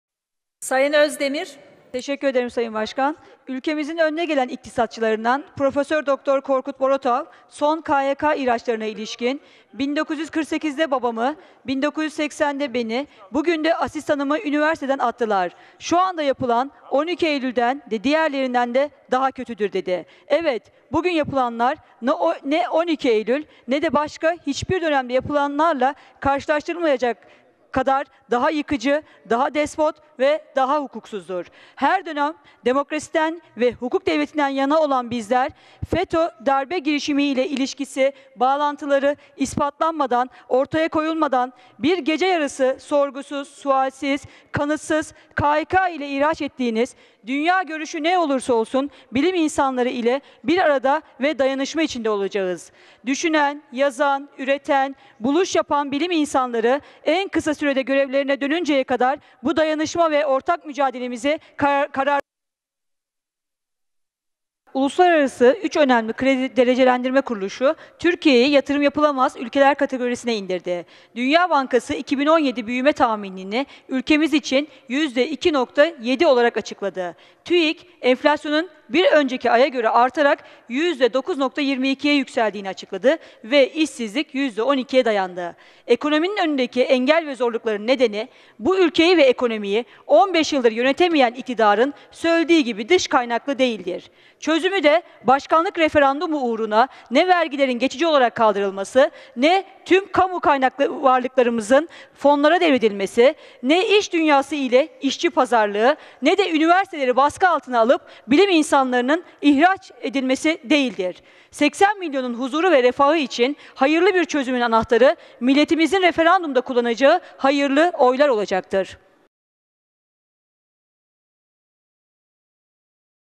Milletin Refahı Ve Huzuru İçin 'Hayır' Diyoruz!
İstanbul Milletvekilimiz Sibel Özdemir; Mecliste, son çıkan KHK'ler ve ülkedeki ekonomik durum hakkında konuştu.